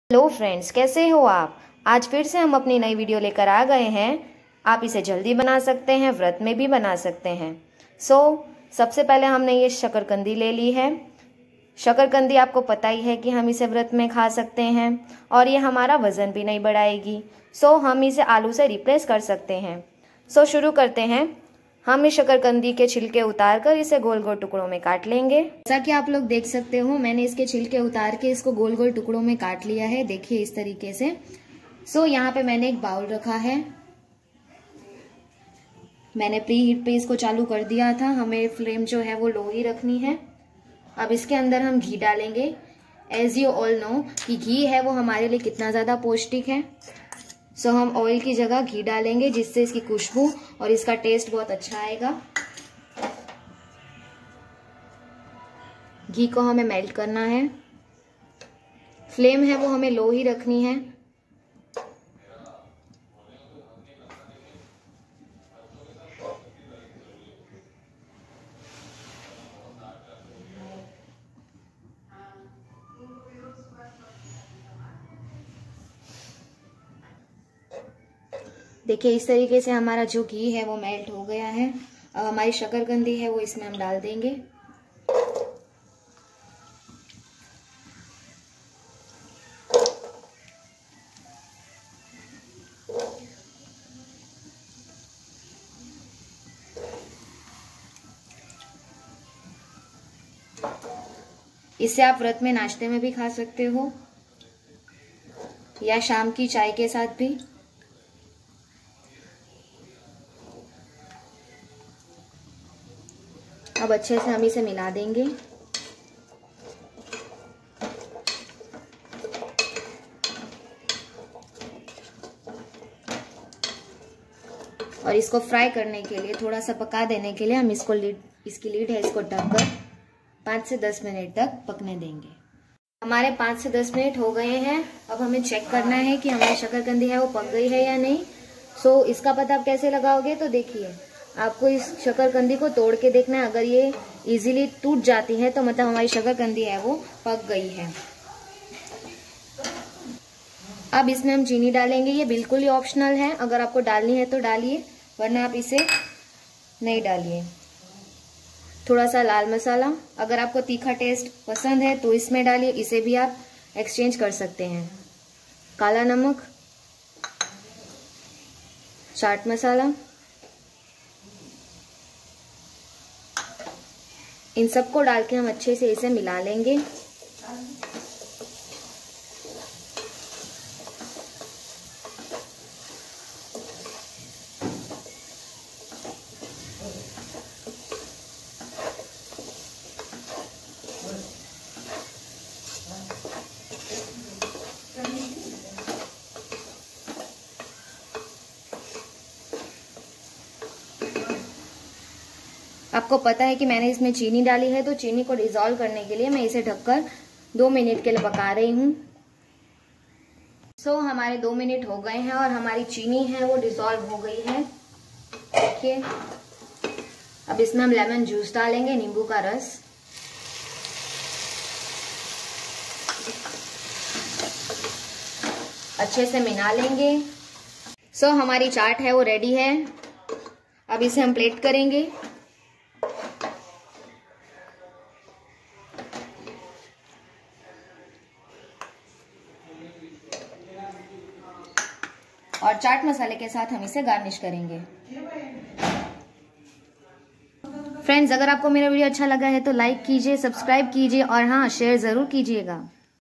Shakarkandi/ Sweet Potato Chaat | शकरकंदी की चाट |Diet Chaat Recipe
Try tasty tangy flavour of this precious weight loss chat.
500g      shakarkandi
2 tbsp    ghee
1tbsp     sugar
1 tbsp    chat masala
1/2 tbsp red chilli powder
1tbsp      lemon juice